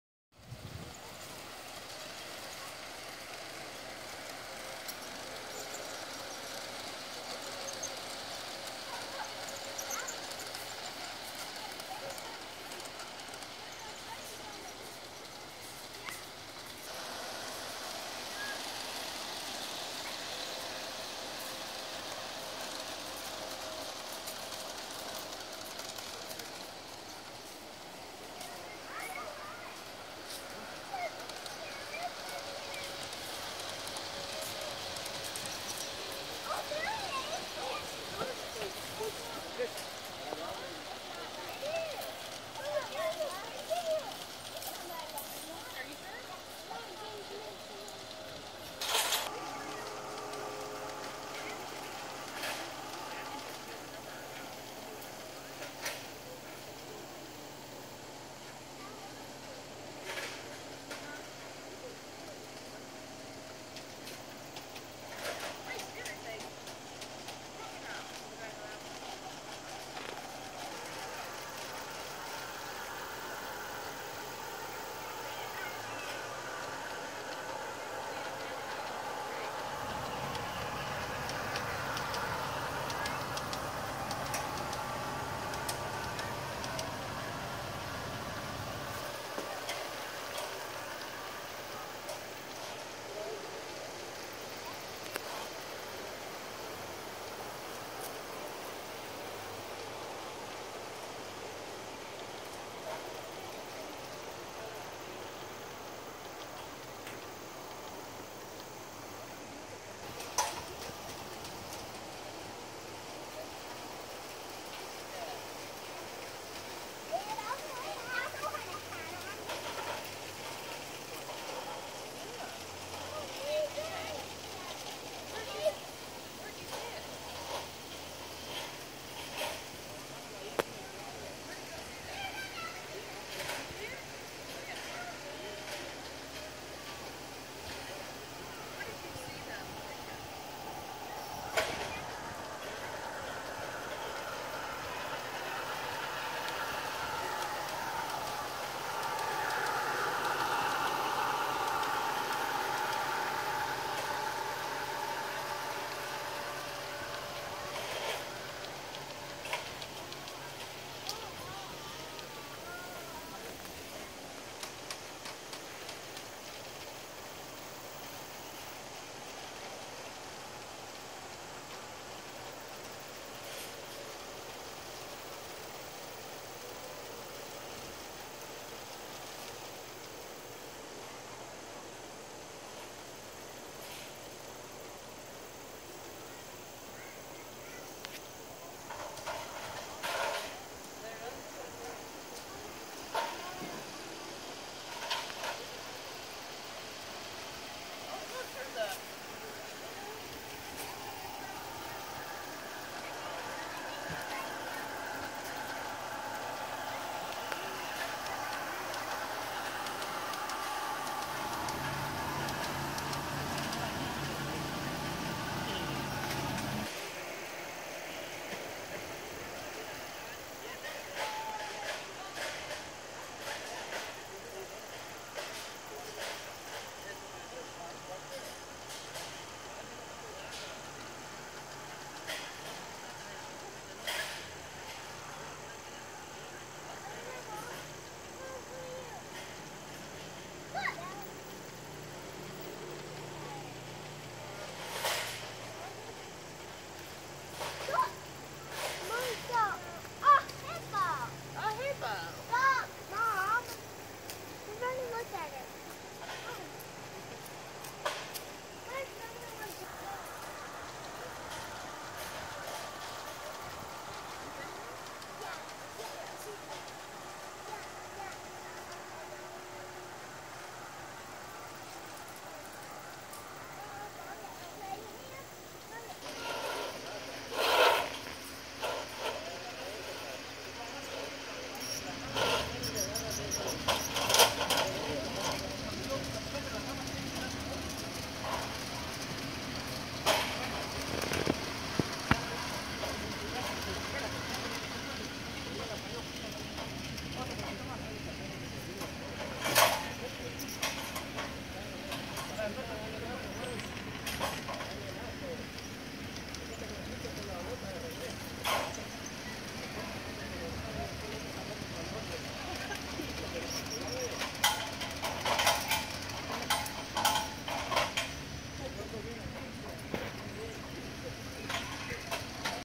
Cheekwood botanical gardens in Nashville Tennessee . Outdoor model railroad train set. 09/27/2017.
Cheekwood botanical gardens in Nashville Tennessee . Outdoor model railroad. O scale train set. September 27th. 2017.